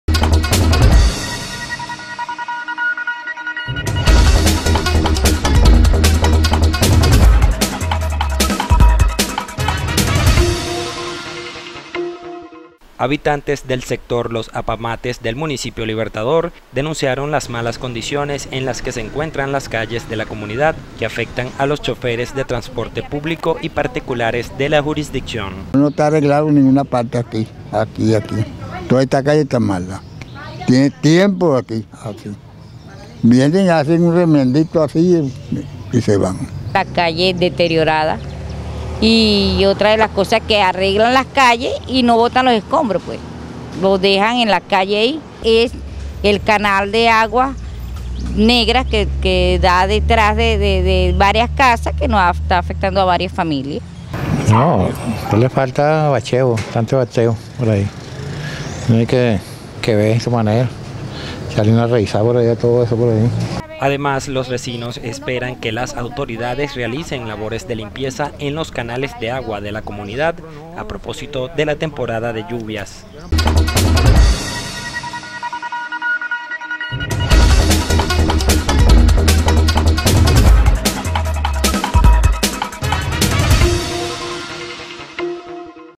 Denuncian malas condiciones de calles en Los Apamates del municipio Libertador 31-07-2014
Habitantes del sector Los Apamates del municipio Libertador denunciaron las malas condiciones en que se encuentran las calles de la comunidad que afectan a los chóferes del transporte público y particulares de la jurisdicción.